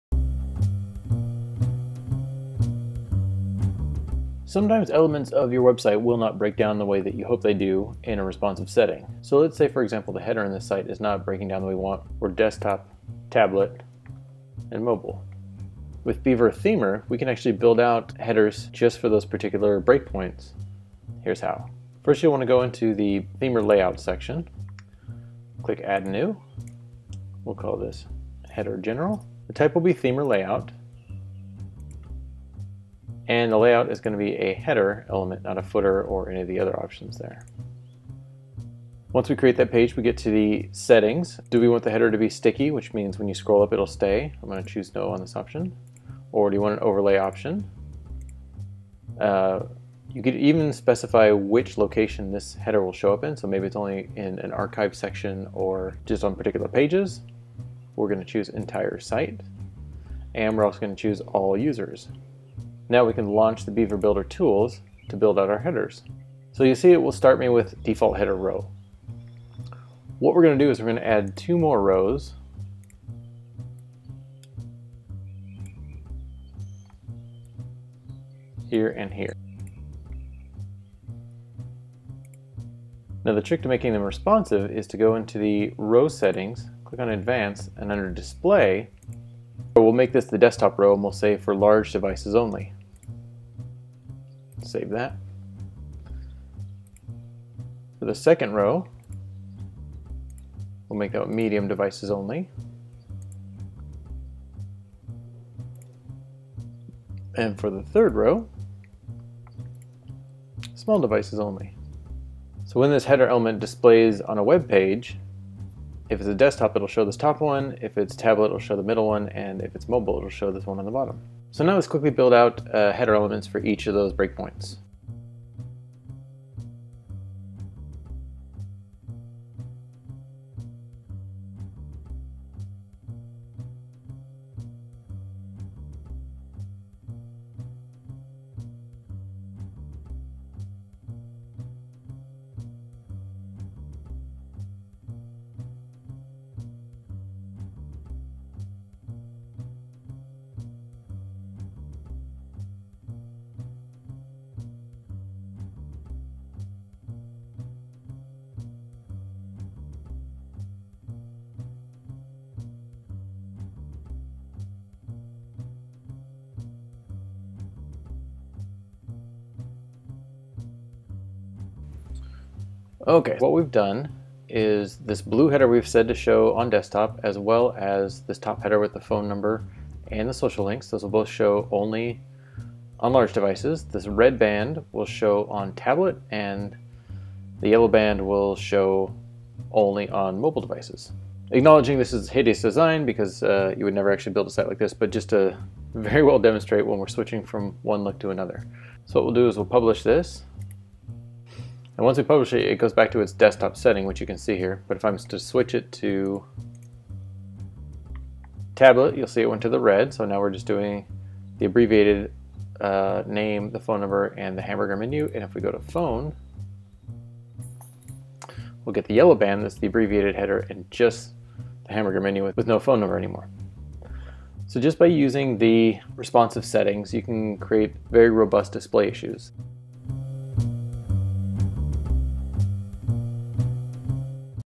Beaver Builder Responsive Headers
With Beaver Builder’s Themer product, you can create layouts, archive pages, headers and footers, and template parts to be automatically applied to your WordPress website. This allows you to create even more customized sites than before. In this video we show off how to create custom responsive headers for three different breakpoints.